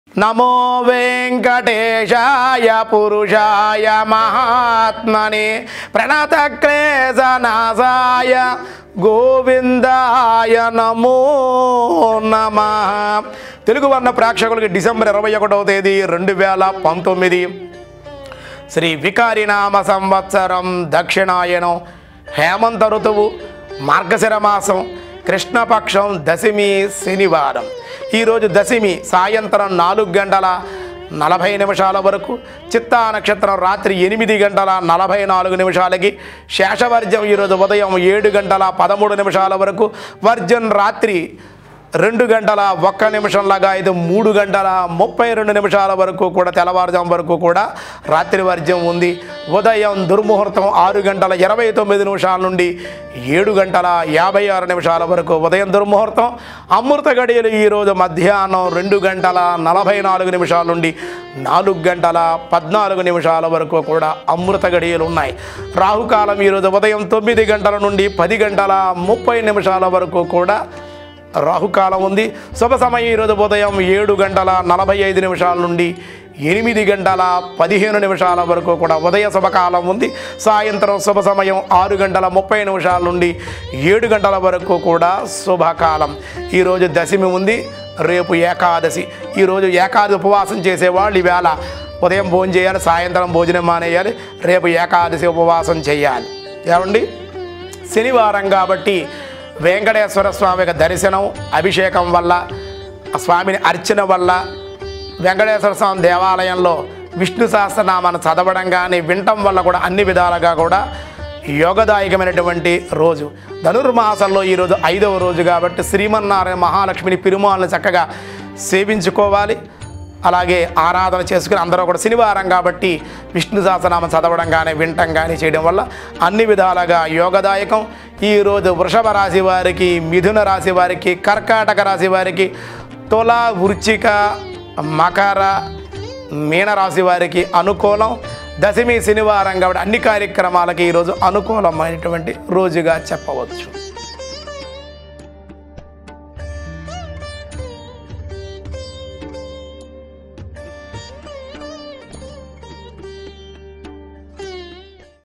Daily Panchangam Telugu | Saturday 21st December 2019 | BhaktiOne
Today Panchangam Saturday 21st December 2019. Watch Daily Panchangam in Telugu with Accurate Muhurtham Timings. DailyPanchangam.